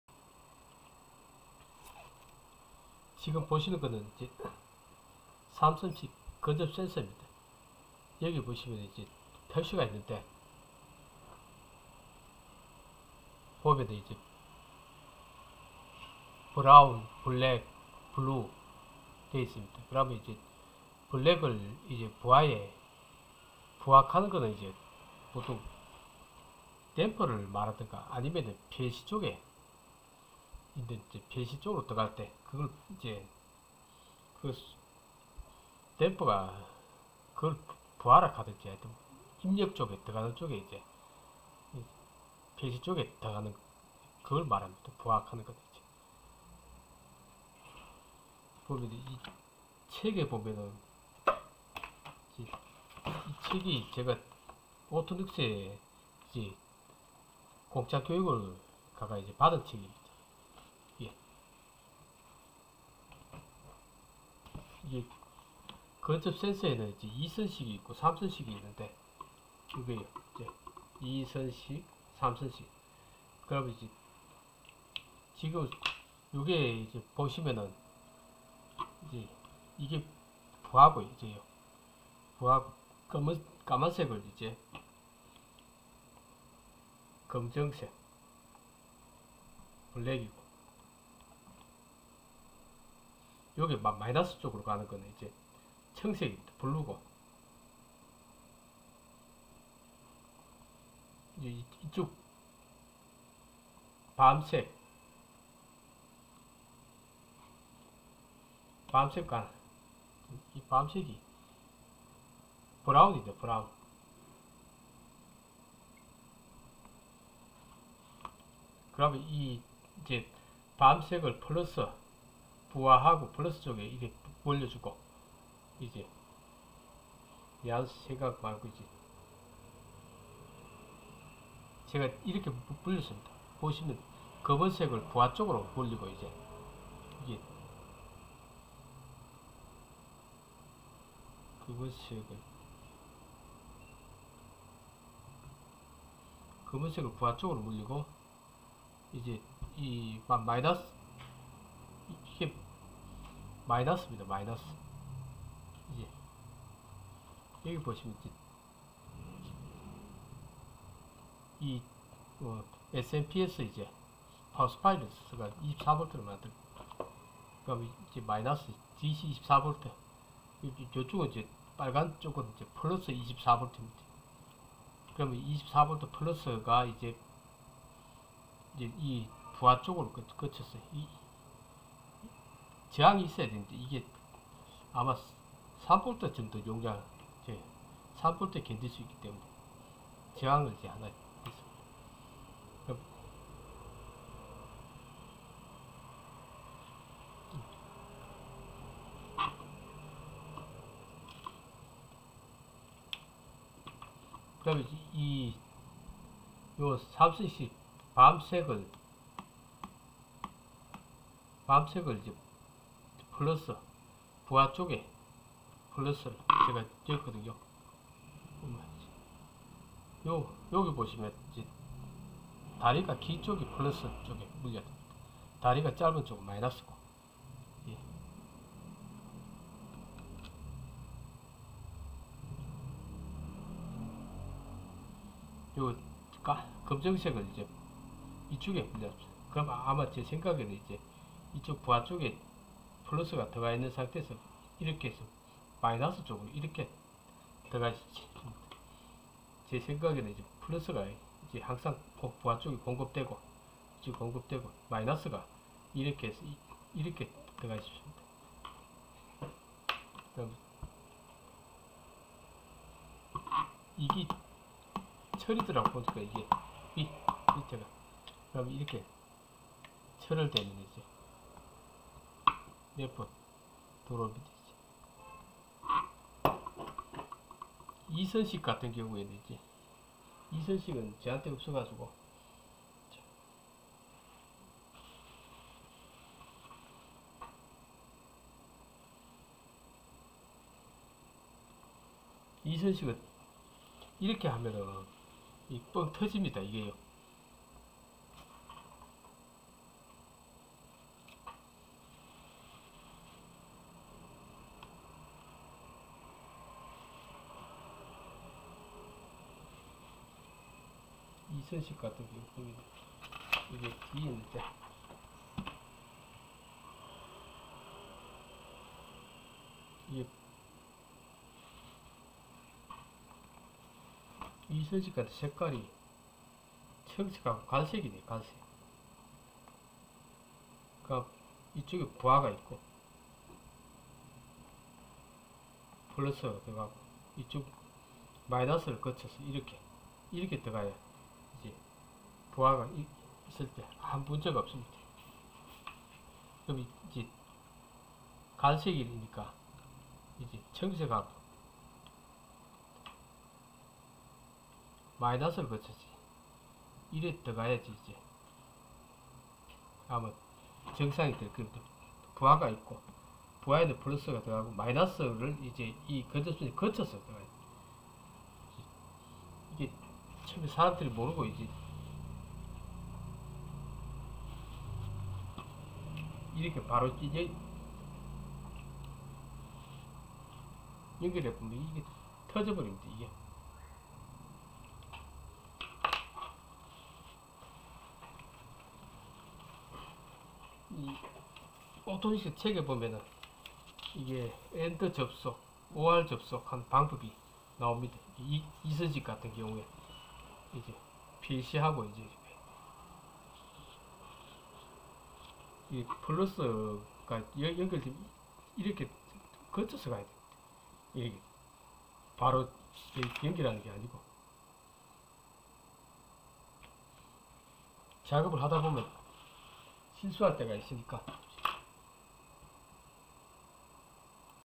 3선식근접센서
3선식 근접센서 가지고 해 본것입니다.